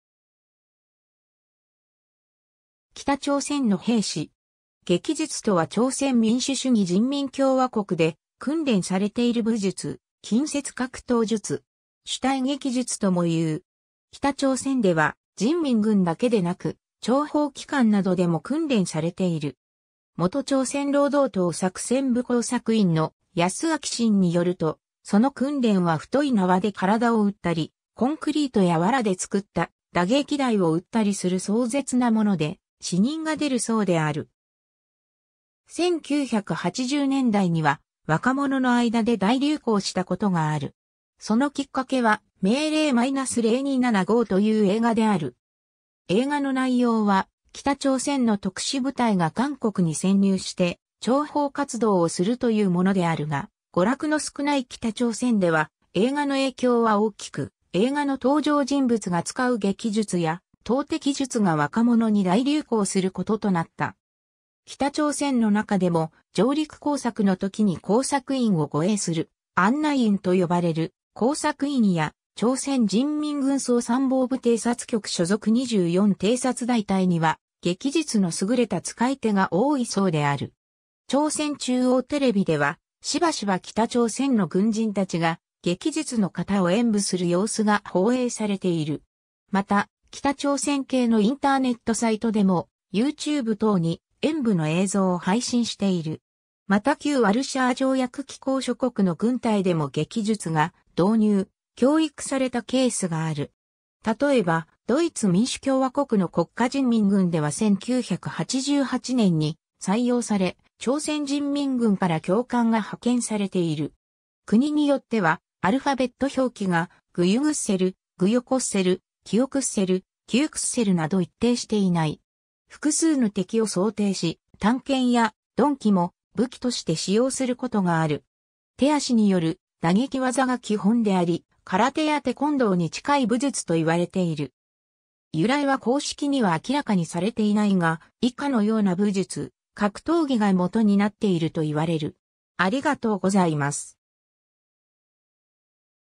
撃術
朝鮮民主主義人民共和国の諜報・情報機関
北朝鮮の兵士。
 撃術（げきじゅつ、朝鮮語　격술、キョクスル、gyeok sul）とは朝鮮民主主義人民共和国（北朝鮮）で訓練されている武術、近接格闘術。
主体撃術（しゅたいげきじゅつ）ともいう。
 北朝鮮では人民軍だけでなく、諜報機関などでも訓練されている。
元朝鮮労働党作戦部工作員の安明進によると、その訓練は太い縄で体を打ったり、コンクリートや藁で作った打撃台を打ったりする壮絶なもので、死人が出るそうである。
 1980年代には若者の間で大流行した事がある。
そのきっかけは「命令－027号」という映画である。
映画の内容は北朝鮮の特殊部隊が韓国に潜入して諜報活動をするというものであるが、娯楽の少ない北朝鮮では映画の影響は大きく、映画の登場人物が使う撃術や投擲術が若者に大流行する事となった。
北朝鮮の中でも、上陸工作の時に工作員を護衛する「案内員」と呼ばれる工作員や、朝鮮人民軍総参謀部偵察局所属24偵察大隊（平壌に駐屯。平壌の緊急時に出動する）には撃術の優れた遣い手が多いそうである。
 朝鮮中央テレビでは、しばしば北朝鮮の軍人たちが撃術の型を演武する様子が放映されている。
また、北朝鮮系のインターネットサイトでも、YouTube等に演武の映像を配信している。
 また旧ワルシャワ条約機構諸国の軍隊でも撃術が導入・教育されたケースがある。
例えばドイツ民主共和国（東ドイツ）の国家人民軍では1988年に採用され、朝鮮人民軍から教官が派遣されている。
国によってはアルファベット表記が Gjogsul、Gyeoksul、Kyŏksul、Kyuksul など一定していない。
 複数の敵を想定し、短剣や鈍器も武器として使用することがある。
手足による打撃技が基本であり、空手やテコンドーに近い武術といわれている。
由来は公式には明らかにされていないが、以下のような武術、格闘技が元になっているといわれる。